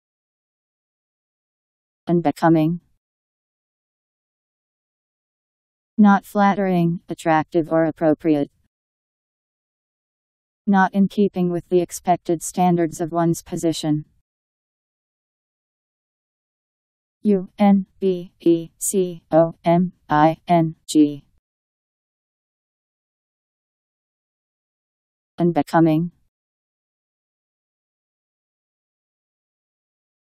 Unbecoming Meaning
Video shows what unbecoming means. Not flattering, attractive or appropriate.. Not in keeping with the expected standards of one's position..  Unbecoming Meaning. How to pronounce, definition audio dictionary. How to say unbecoming. Powered by MaryTTS, Wiktionary